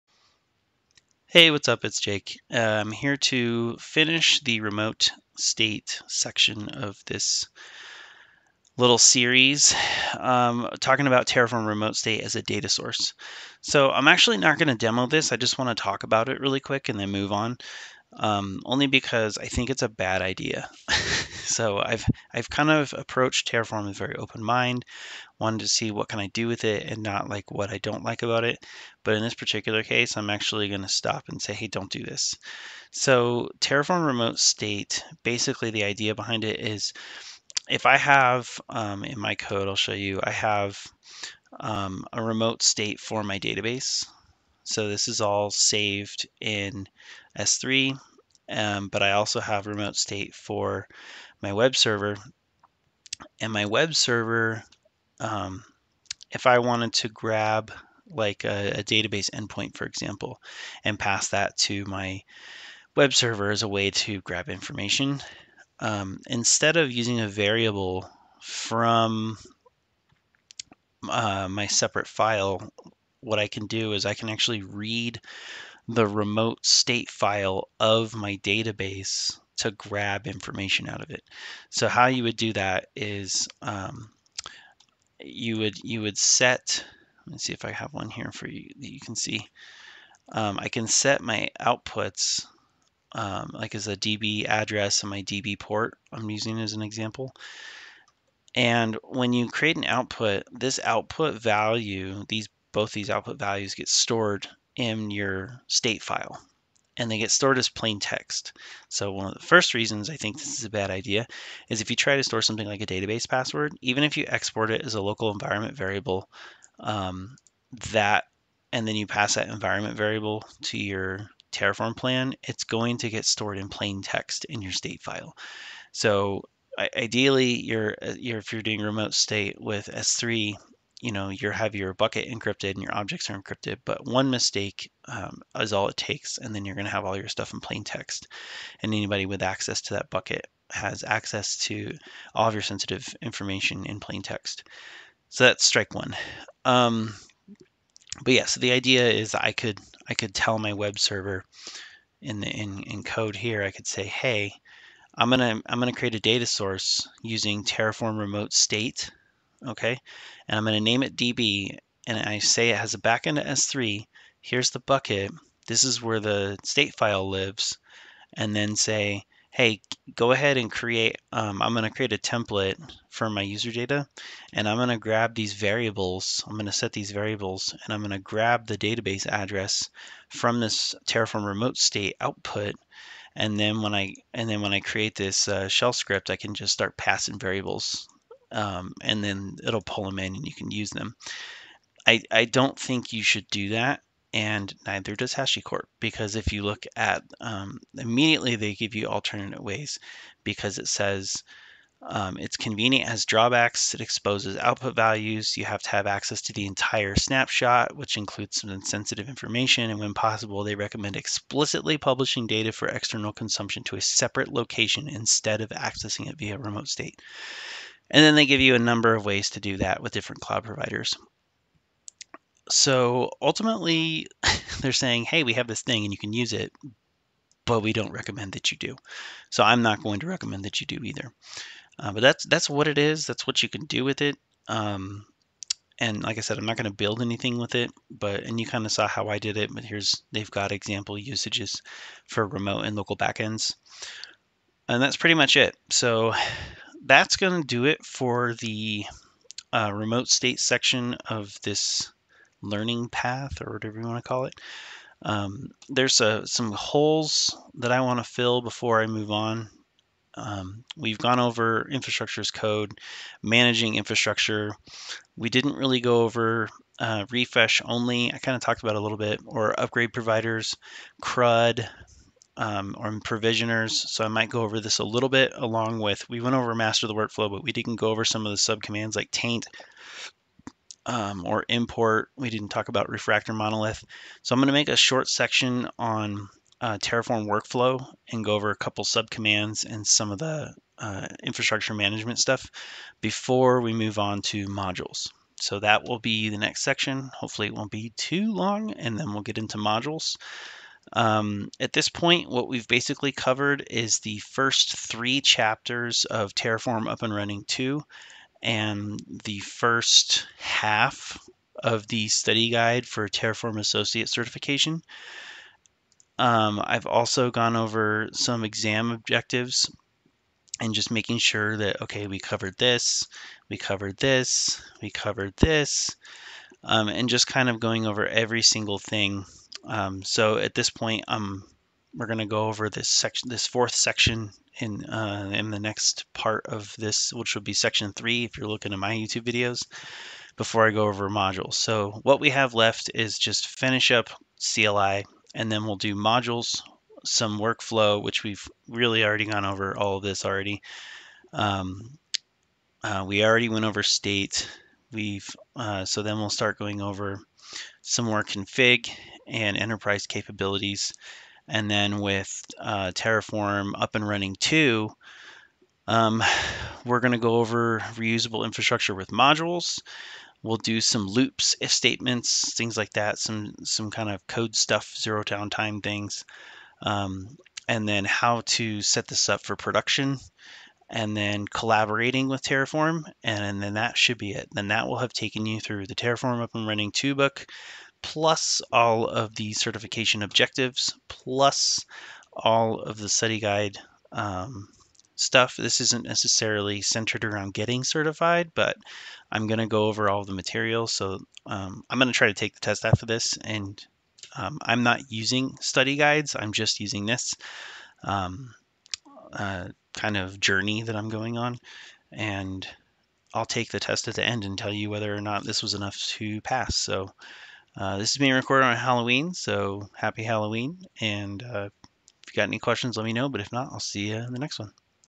2.5 Learn Terraform - Remote State Data Source... Bad Idea
We don't build anything in this video but I just go over the theory of terraform_remote_state as a data source. I chose this approach because it's just a really bad idea and I don't want to teach anyone any bad habits (including myself). We close this chapter on remote state and talk about where we're going next.